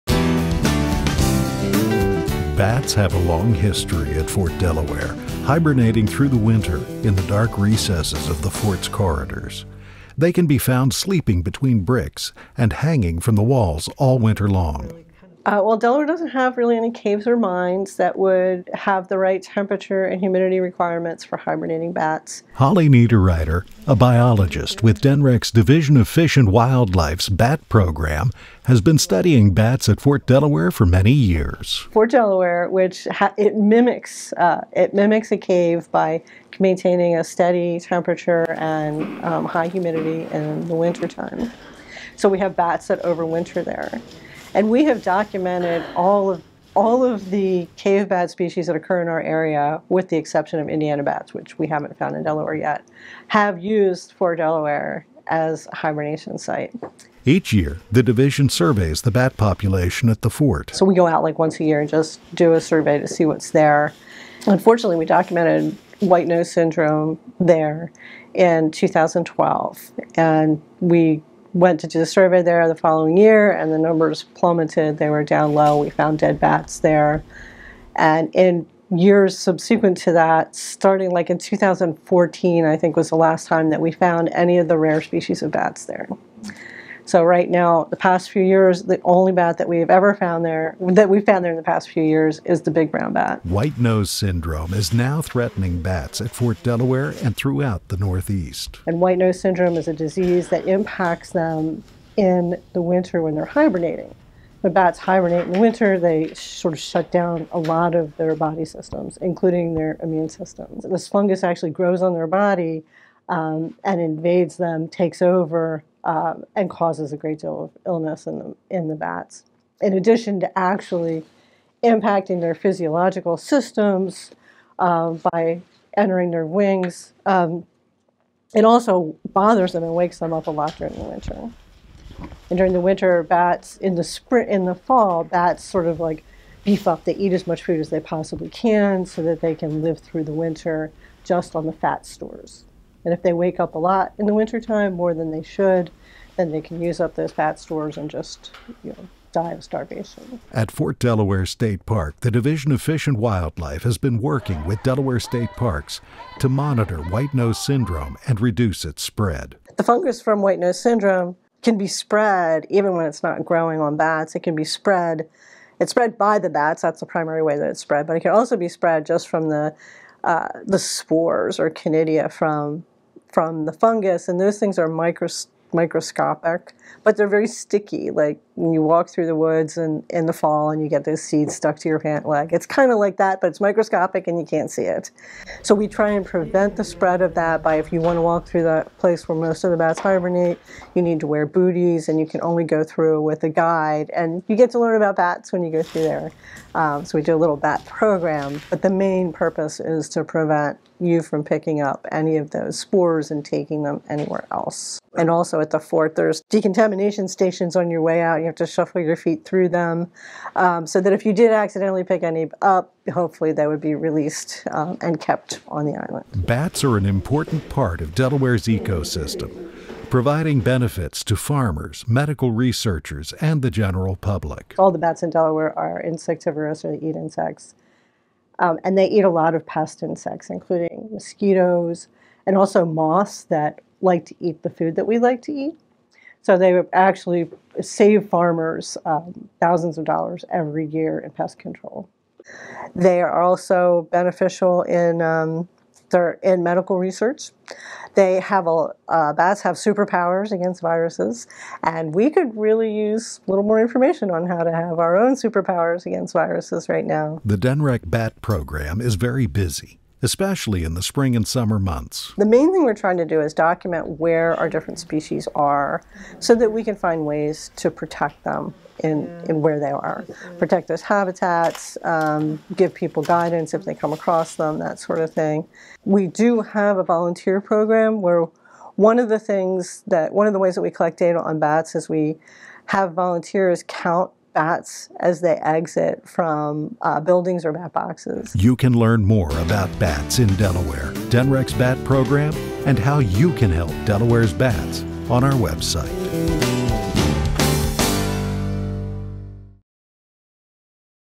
Fort Delaware Bats and White Nose Syndrome
Bats are one of the most mysterious and least understood groups of mammals. Discover the bats of Fort Delaware; the species we have, how to attract or safely evict them, get information about White-Nose Syndrome, find out what the state is doing for bats, and how you can help.